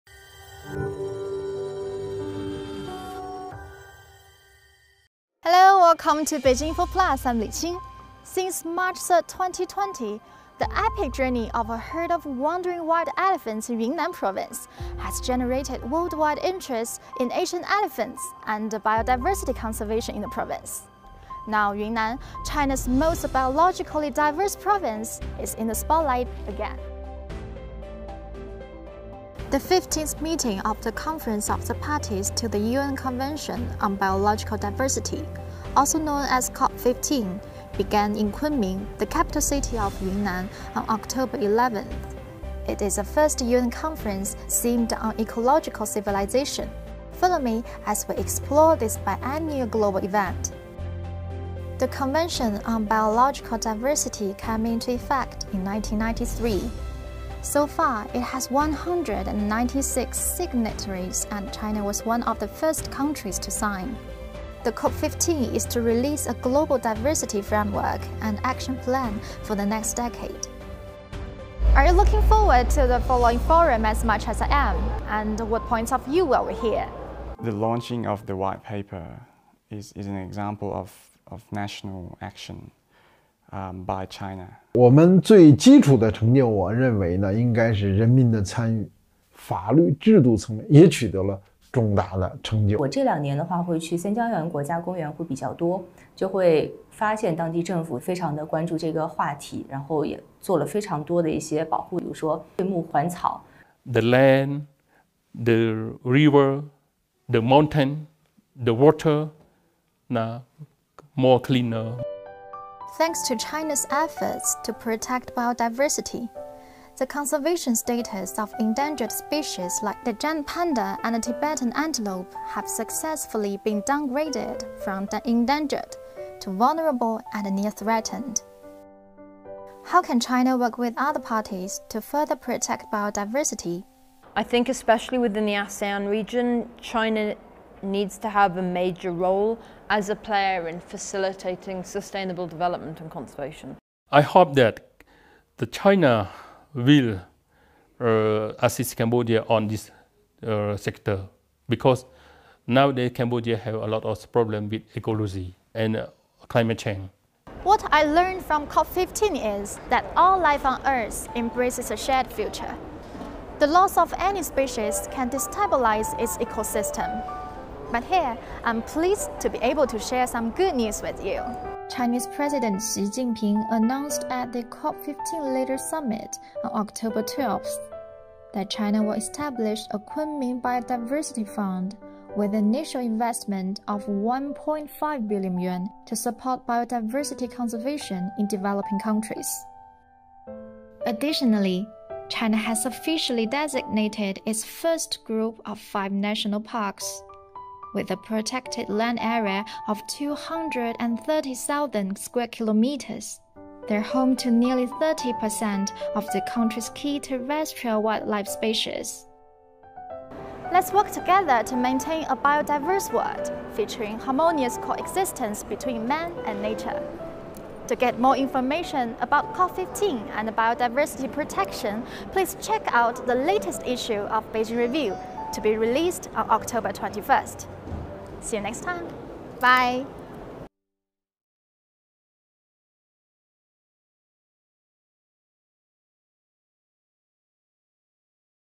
Beijing Review on COP15|All Life on Earth Embraces a Shared Future
All life on Earth embraces a shared future. The loss of any species can destabilize its ecosystem. Chinese President Xi Jinping announced that China will establish a Kunming Biodiversity Fund with an initial investment of 1.5 billion yuan to support biodiversity conservation in developing countries.
For more, please follow her to explore this biennial global event.